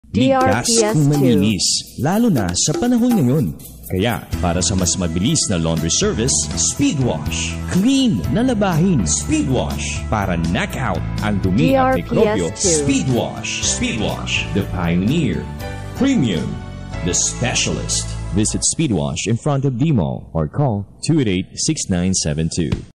Speedwash Laundry Shop Radio Ad 2022-2023 23s (Boracay)
Ito po ang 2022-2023 Radio Ad ng Speedwash Laundry Shop sa Boracay.

DRPS Two 3895: Speedwash Laundry Shop Radio Ad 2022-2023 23s (Boracay)